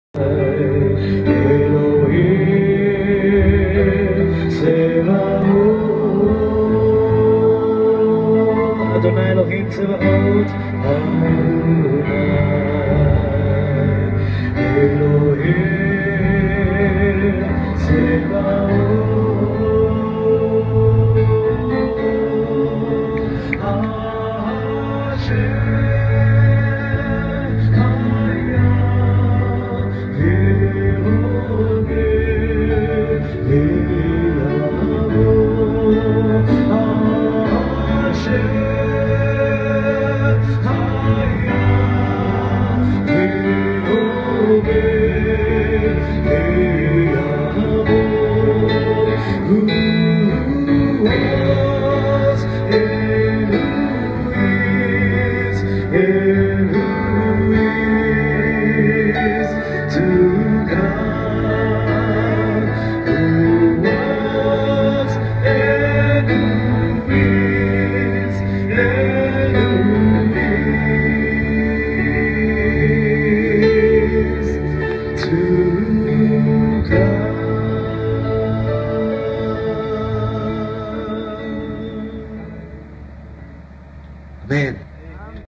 Worship on the Galilee with Daniel Carmel
Worship on the Galilee with Daniel Carmel on a Zola Levitt Ministries Tour.